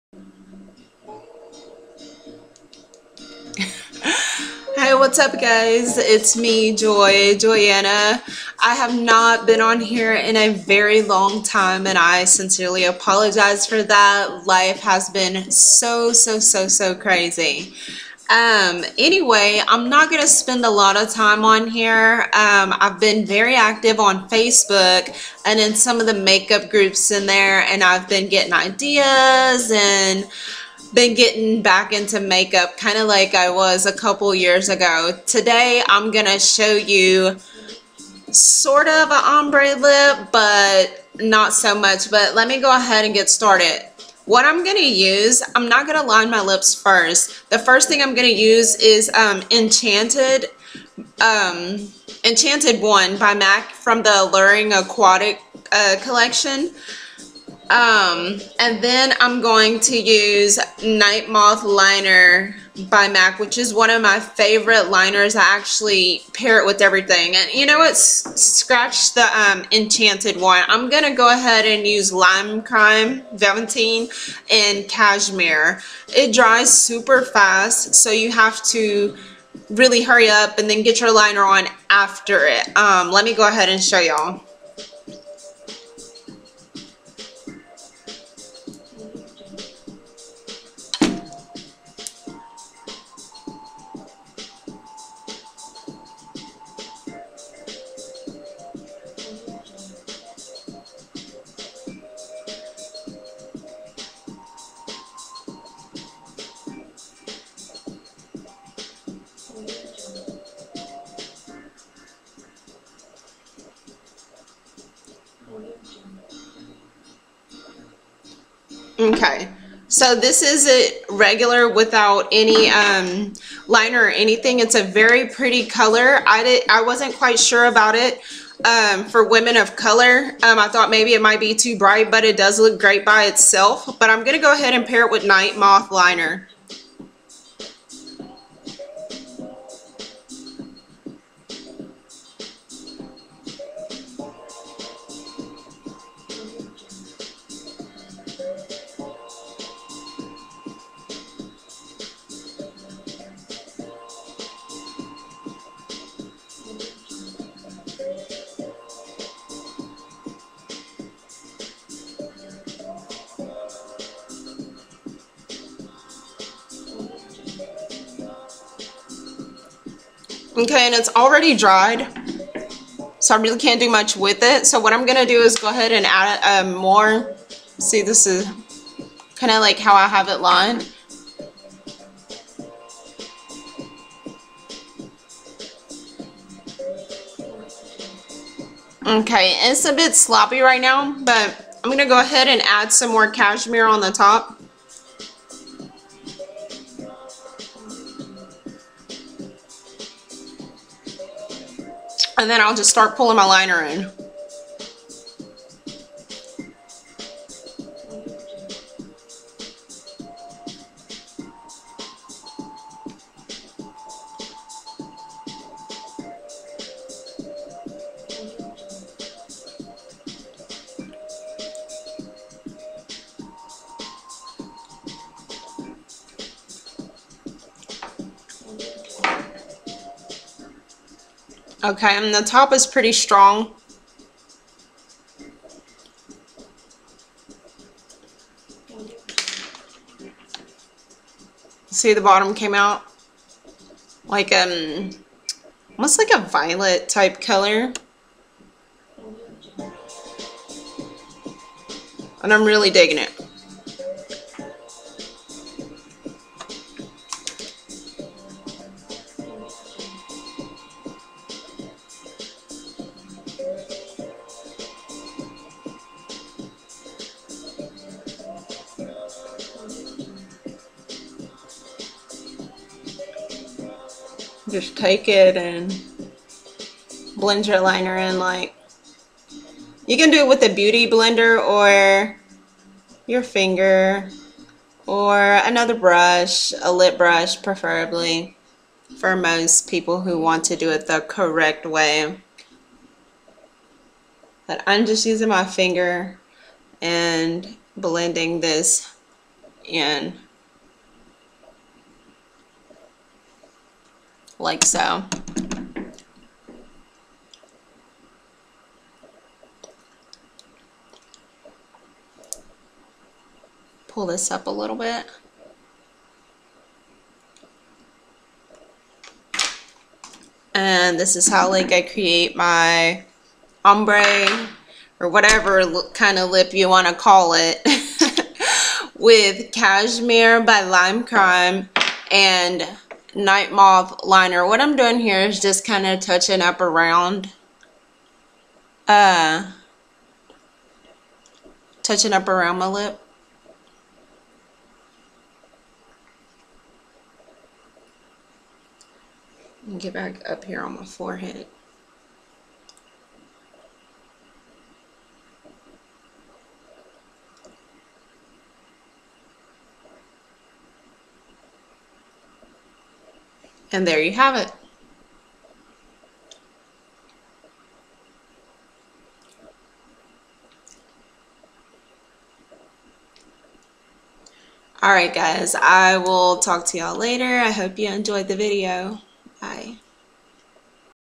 Easy Ombre Limecrime Cashmere and MAC Nightmoth
Limecrime Cashmere Velvintine and Nightmoth liner

At the moment Limecrime is sold out of cashmere but you can find other beautiful shades here: